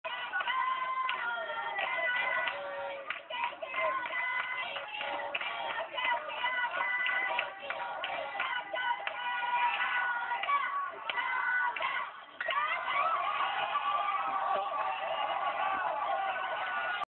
中学時代の山田陽太 ライトＨＲ
中学時代の山田陽太のホームランの瞬間です